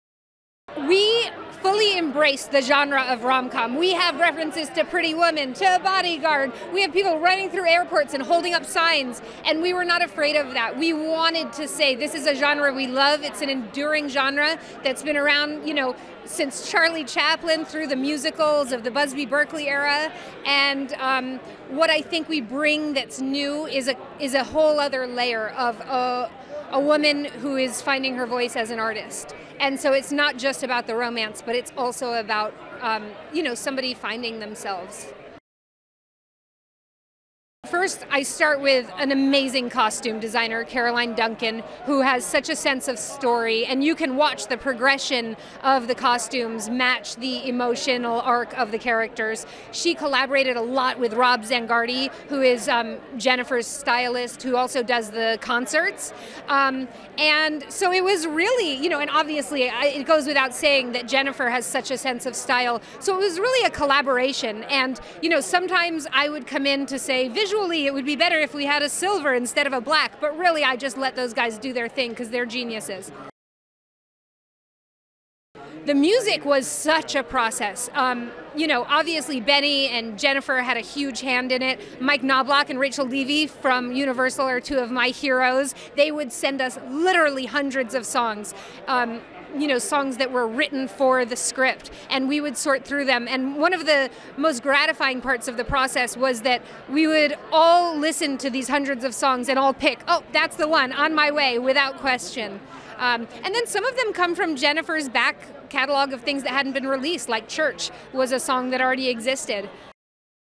'Marry Me' Los Angeles Special Screening: An Interview with Director Kat Coiro
URHere with Director Kat Coiro at the Los Angeles Special Screening of "Marry Me." "Marry Me" opens in theaters on February 11th.